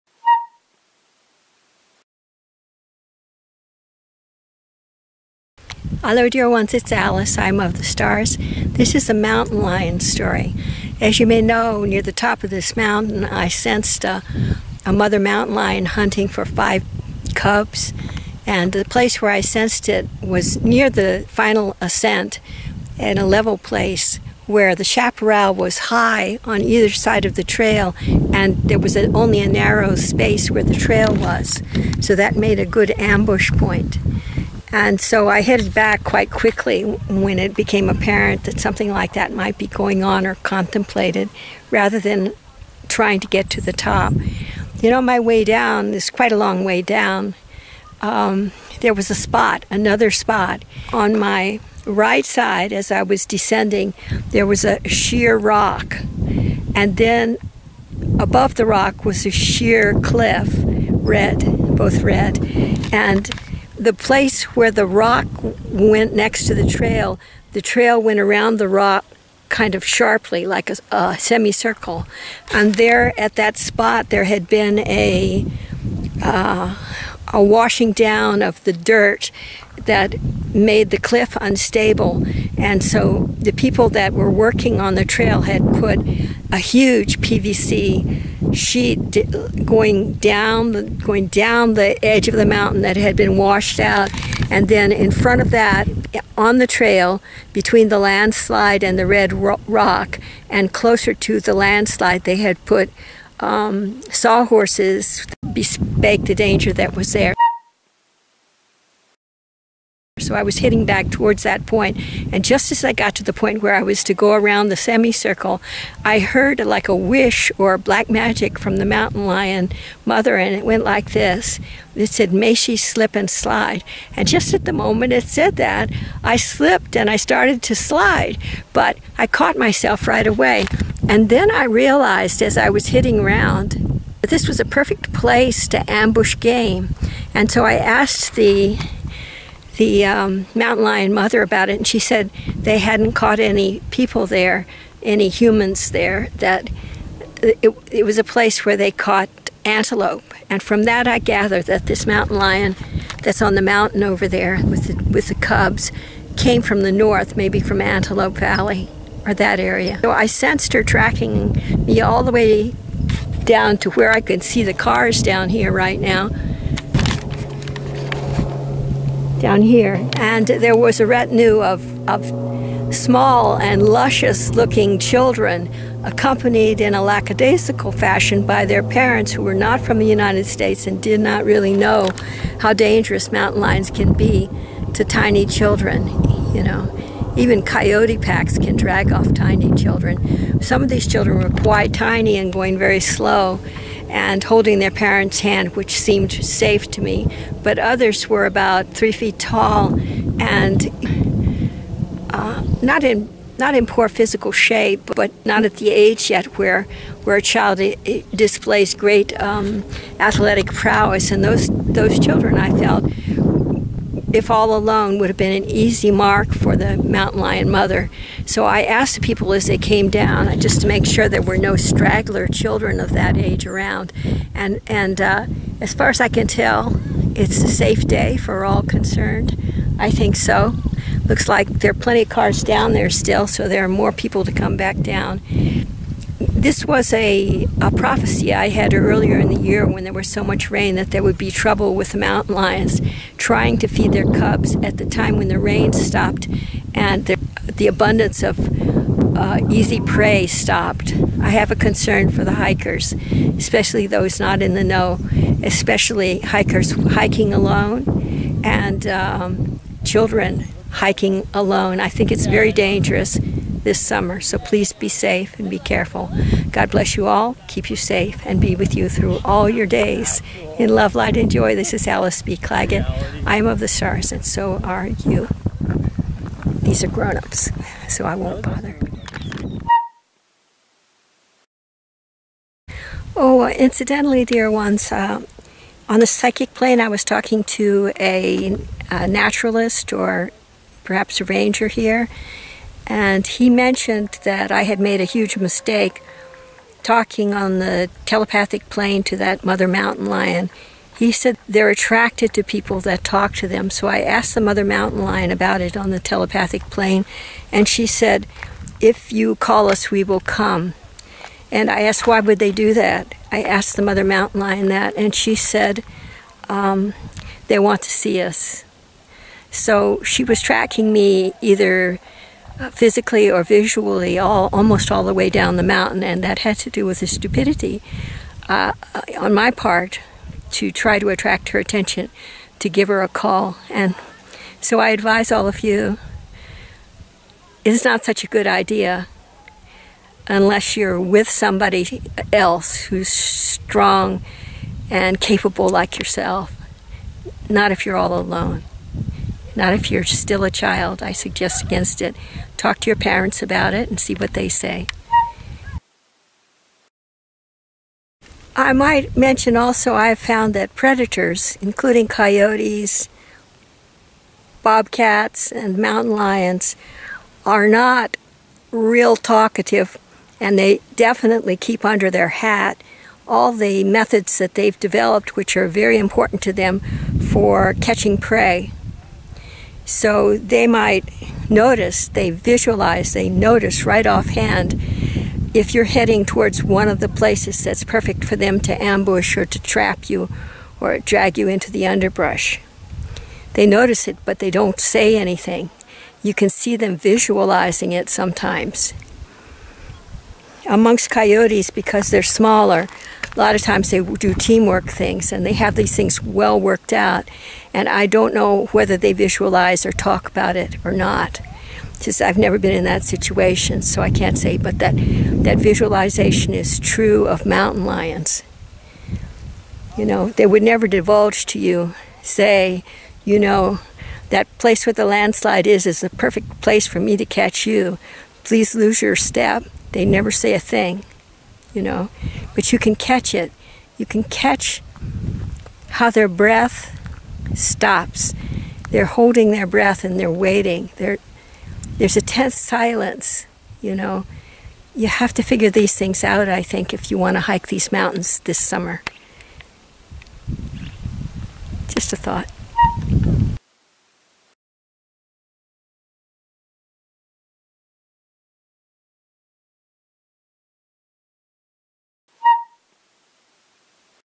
Voice of Mother Mountain Lion 2 . Don't Talk to Mountain Lions! . Coyote and Mountain Lion Lingo
FULL TITLE: FEATURE: Voice of Mother Mountain Lion 2  SHORT: I Am Advised Not to Talk to Mountain Lions!  SHORT: Coyote, Bobcat, and Mountain Lion Lingo . by Alice B. Clagett

Filmed on 19 June 2024 and published on 4 July 2024

FEATURE: Voice of Mother Mountain Lion 2  SHORT: I Am Advised Not to Talk to Mountain Lions!  SHORT: Coyote, Bobcat, and Mountain Lion Lingo

Filmed on 19 June 2024 and produced on 4 July 2024

Location: Santa Monica Mountains, CA